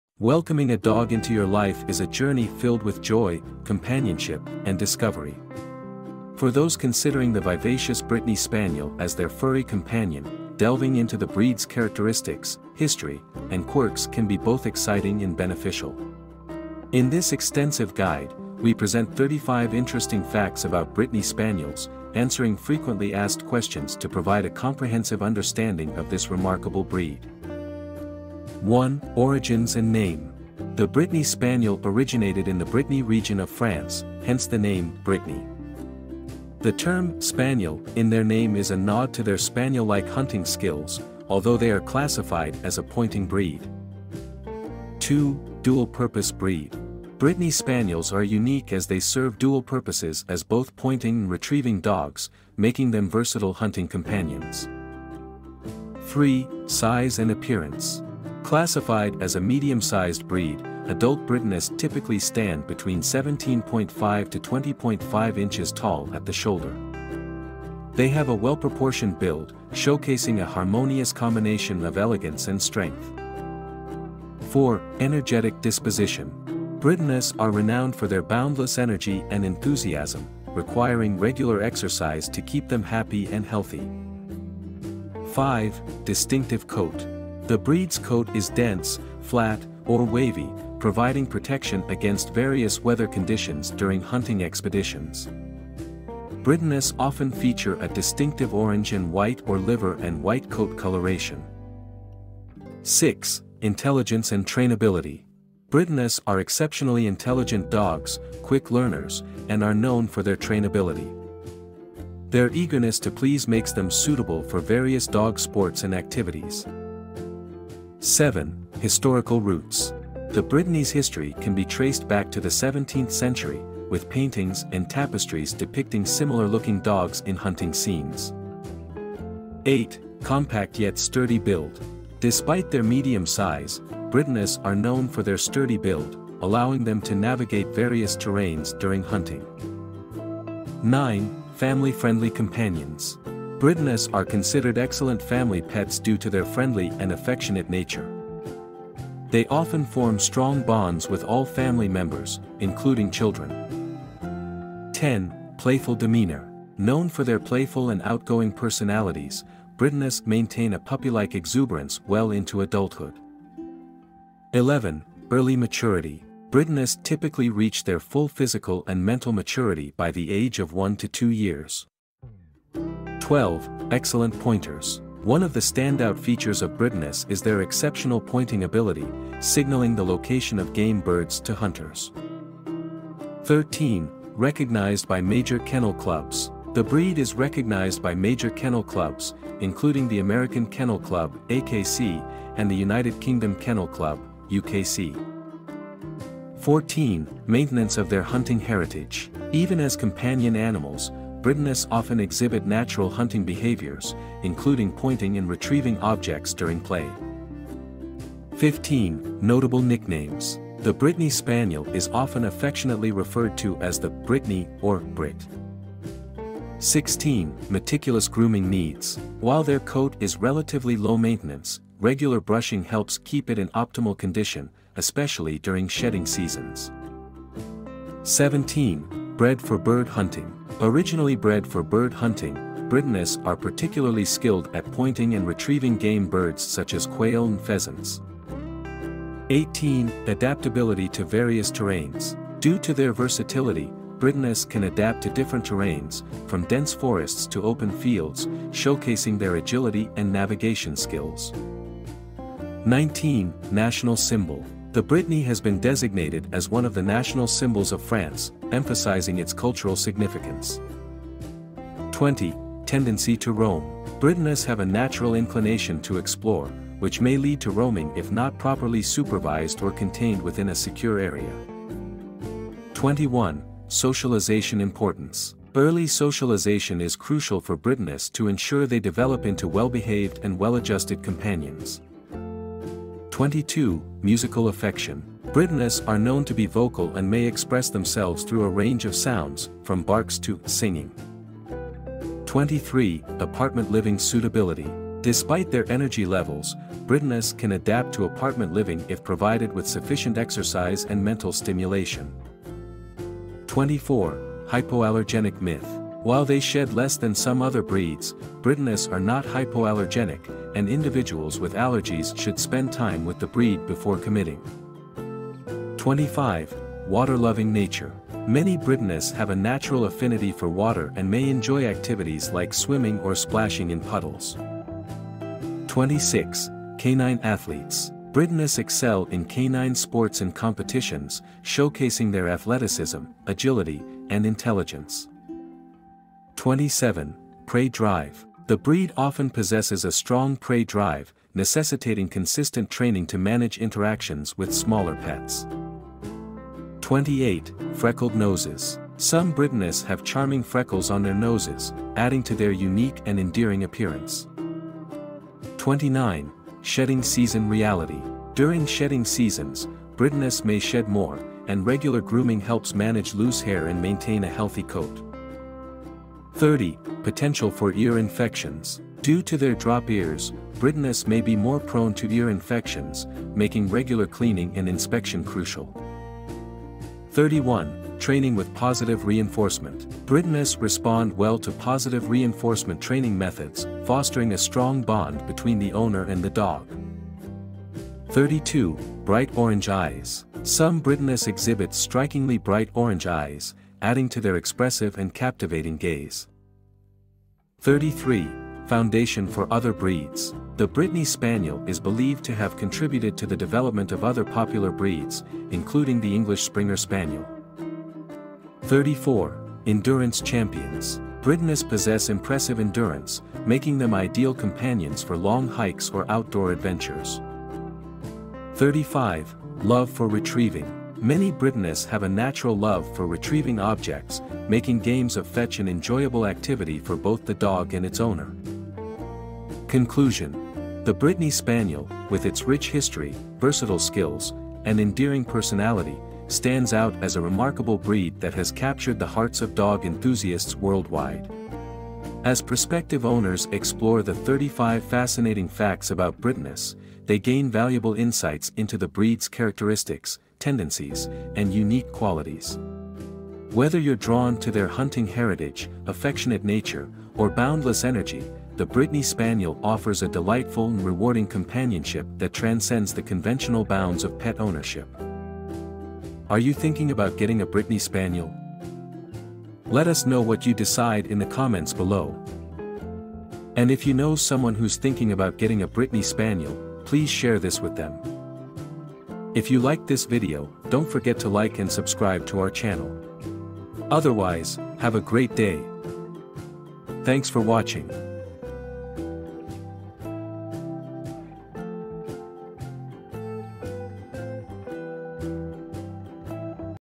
35 Interesting Facts about Brittany Spaniels (FAQs)🐶🐶😍😎
🐶 Calling all dog enthusiasts and Brittany Spaniel lovers! In this captivating video, we're diving into the world of Brittany Spaniels, unveiling 35 interesting facts that will make you fall in love with this charismatic breed. Whether you're a potential owner or simply fascinated by canine companions, join us on a journey of discovery as we explore the unique qualities, historical roots, and endearing quirks of Brittany Spaniels.

🌟 Video Highlights:

Dual-Purpose Prowess: Discover why Brittany Spaniels are celebrated for their dual-purpose capabilities as both pointers and retrievers.
Historical Roots: Uncover the rich history of the Brittany Spaniel, dating back to the 17th century with depictions in paintings and tapestries.
Distinctive Name Origin: Learn why the breed is named after the Brittany region in France and the significance of the term "Spaniel" in their name.
Medium-Sized Marvels: Explore the moderate size and well-proportioned build that contribute to the Brittany's agility and adaptability.
Energetic Disposition: Understand the boundless energy and exuberance that characterize the Brittany Spaniel, making them ideal companions for active individuals.
Versatile Hunting Skills: Delve into the breed's remarkable hunting instincts, excelling in pointing and retrieving game birds like quail and pheasants.
Brittany Spaniel Colors: Admire the distinctive coat colors, including orange and white or liver and white, that add to the breed's visual charm.
National Symbol of France: Learn about the cultural significance of the Brittany Spaniel, designated as one of the national symbols of France.
Playful Demeanor: Witness the playful and outgoing nature of Brittanys, maintaining a puppy-like exuberance well into adulthood.
Musical Affection: Explore their vocal expressions, from barks to "singing," as Brittanys communicate their feelings.
Love for Water: Discover the natural affinity many Brittanys have for water, often enjoying activities like swimming and splashing.
Notable Nicknames: Hear endearing nicknames such as "Britt" affectionately used for these charming dogs.
Dual Heritage Contributions: Understand how Brittany Spaniels have contributed to the development of other popular breeds, including the English Springer Spaniel.
Endearing Freckles: Admire the charming freckles that some Brittanys sport on their noses, adding to their unique appearance.
Bright Orange Eyes: Explore the captivating gaze of Brittanys, with some individuals featuring strikingly bright orange eyes.
Ear Infection Awareness: Recognize the breed's susceptibility to ear infections due to their drop ears, emphasizing the importance of regular cleaning.
National Symbol Status: Appreciate the Brittany's historical recognition as a national symbol, highlighting its importance in French culture.
Compact Yet Sturdy Build: Understand how their medium size conceals a sturdy build, allowing Brittanys to navigate various terrains during hunting.
Intelligence and Trainability: Discover the remarkable intelligence and trainability that make Brittanys quick learners, excelling in various training activities.
Brittany Spaniel Origins: Explore the Brittany's roots in bird hunting and its evolution into a beloved family companion.
Bright Eyes and Expressive Faces: Admire the expressive faces of Brittanys, coupled with bright eyes that convey their emotions with charm.
Brittany Puppies' Journey: Witness the journey from adorable Brittany Spaniel puppies to the energetic and affectionate adults they become.
Sociable Nature: Learn about the sociable nature of Brittanys, making them not only loving family members but also friendly with other pets.
Prey Drive and Training: Understand the breed's strong prey drive and the importance of consistent training to manage interactions with smaller pets.
Brittany Spaniel Endurance: Marvel at their impressive endurance, solidifying their status as ideal companions for long hikes and outdoor adventures.
Doggy Artistry: Enjoy artistic portrayals, illustrations, and creative expressions celebrating the beauty and charm of the Brittany Spaniel.
Adaptability to Apartment Living: Explore how Brittanys can adapt to apartment living with sufficient exercise and mental stimulation.
Brittany Companionship Chronicles: Hear heartwarming stories from Brittany owners about the special bond they share with their furry friends.
Grooming Guide.
Canine Athletes Unleashed.
Brittany Spaniel Club Community.
Brittany Spaniel Training Triumphs.
Health and Wellness Wisdom.
Not Hypoallergenic Reality.
Brittany Spaniel Magic.

🌈 Whether you're a seasoned Brittany owner or simply captivated by canine charisma, this video promises a delightful exploration of the Brittany Spaniel's world. 🐾💖 BrittanySpaniel DogFacts CanineCharm 🏡🐶